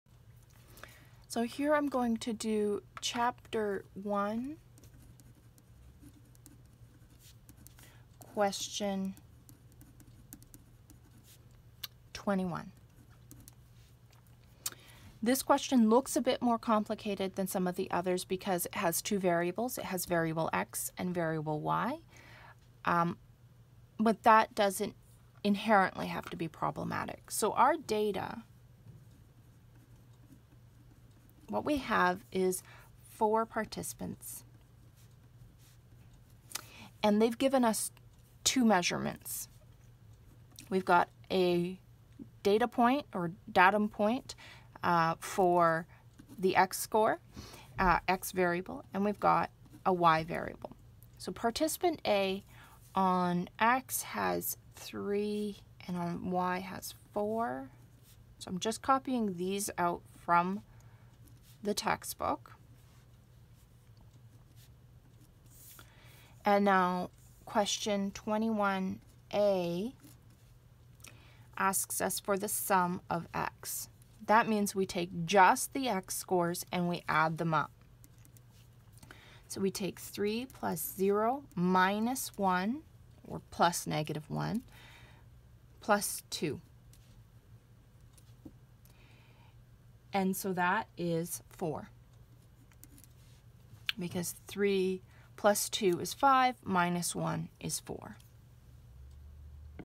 Statistical Notation - Order of Operations - Chapter 1 Question 21a
Chapter 1 Question 21a

Gravetter & Wallnau - Cengage - Statistics for the Behaivoral Sciences 10th ed